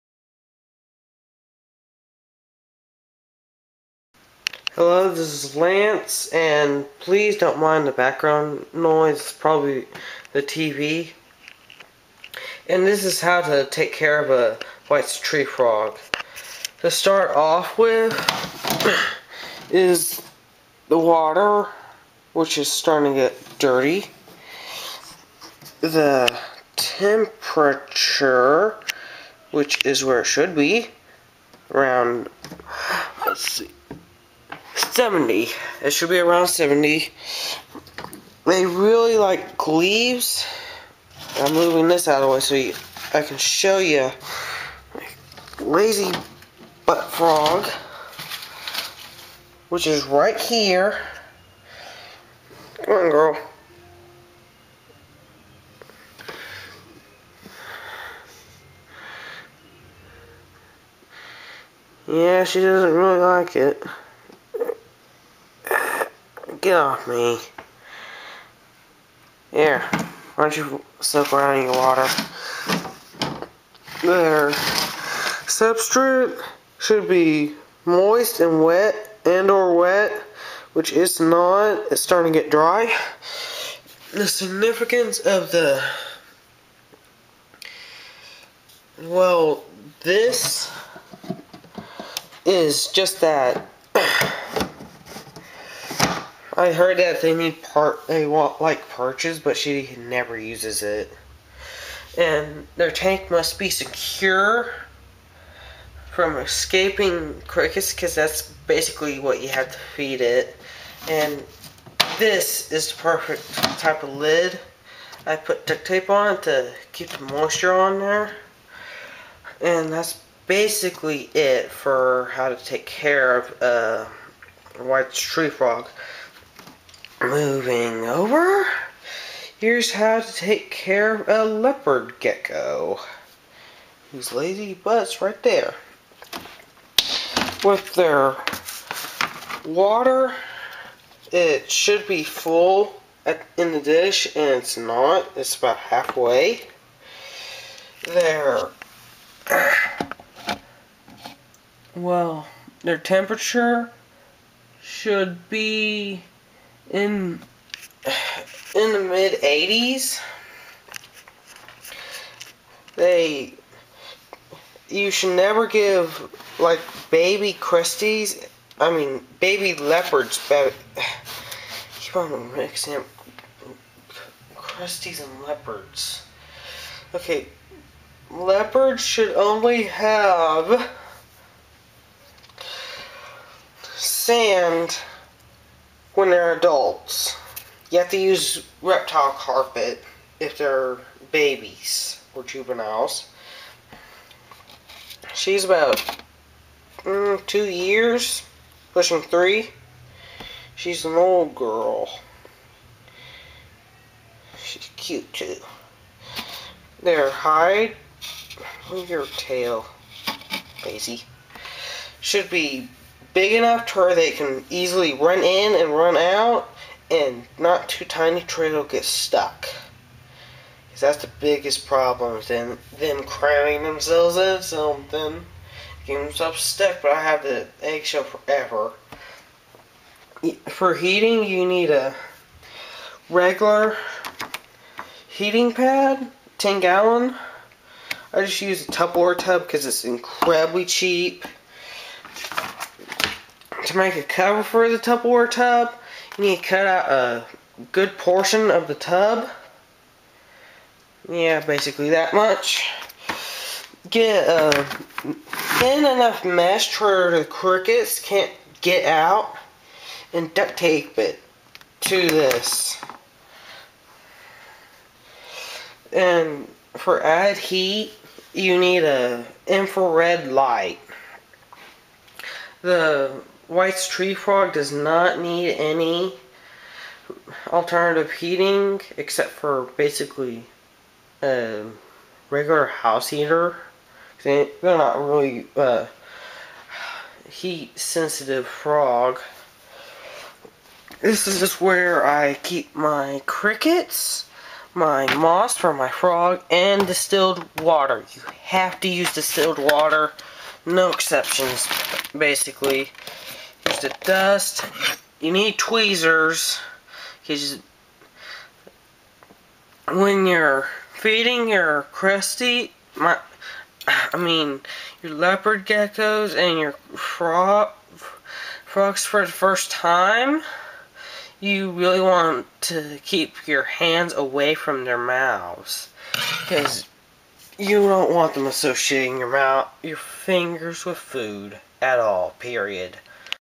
how to take care of a leopard gecko and white's tree frog
part 1 of my introductory speech